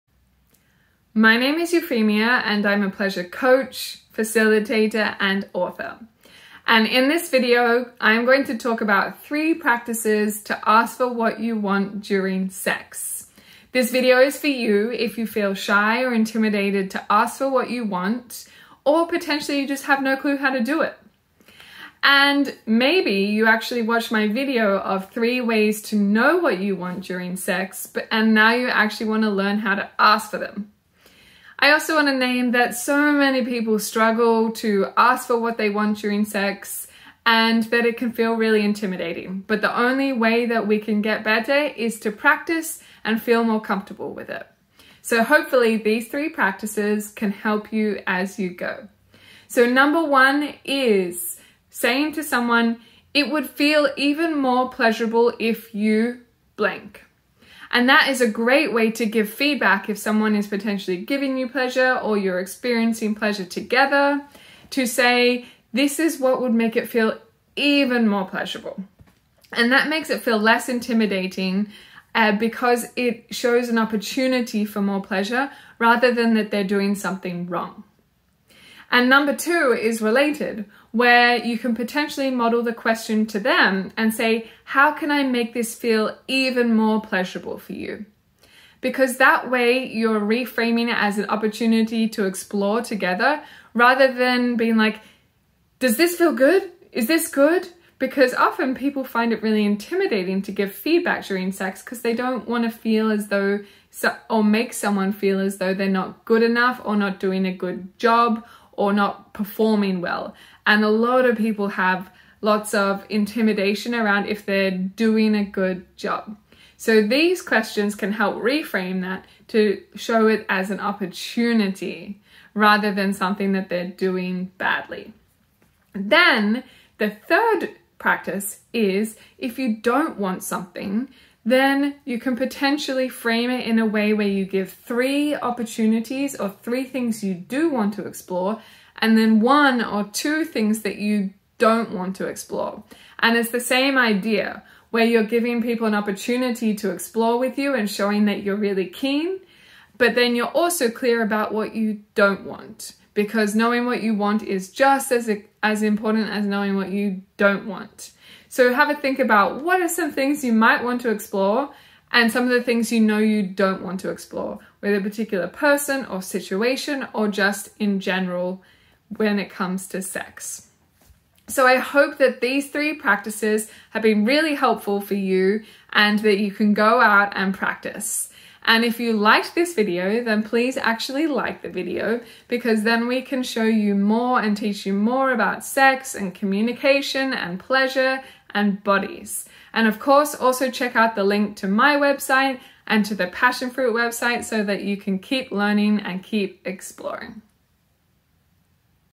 3 Ways To Ask For What You Want In Bed
Euphemia gives their Top 3 Tips on ASKING FOR WHAT YOU WANT IN BED

🏠Chat with us in store/online to get matchmade to your perfect vibe:
PASSIONFRUIT: 404, Bridge Road, Richmond, Melbourne.